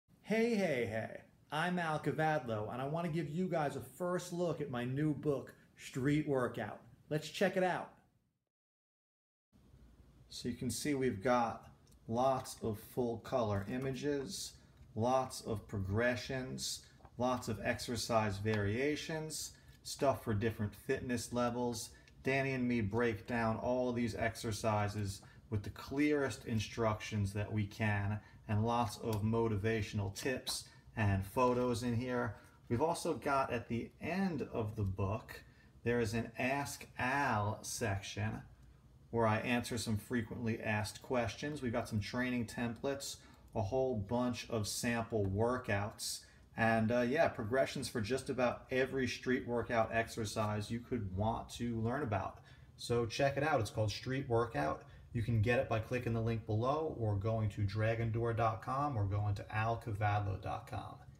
Street Workout First Look
Al Kavadlo gives viewers a sneak peek inside his new book Street Workout!